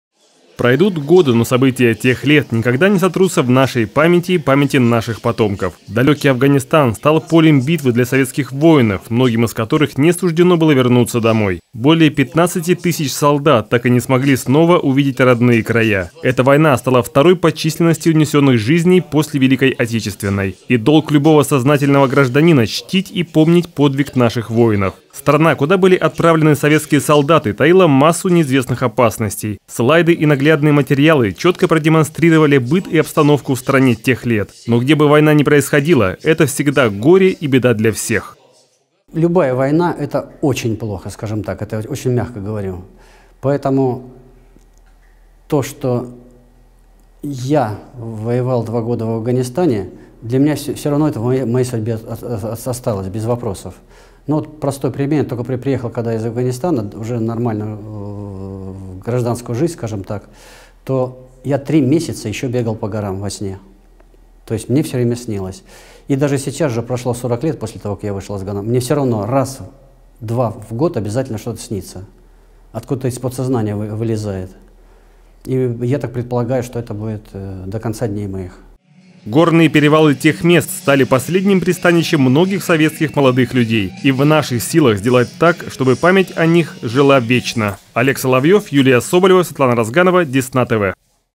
Десна-ТВ: «Памяти героев»: участники боевых действий в Афганистане рассказали о ужасах войны
В преддверии 30-летия вывода войск из Афганистана в выставочном зале «Феникс» прошел урок мужества. Беседу с гостями провел офицер Игорь Журавлев, руководитель городской ассоциации ветеранов. Подробности в сюжете.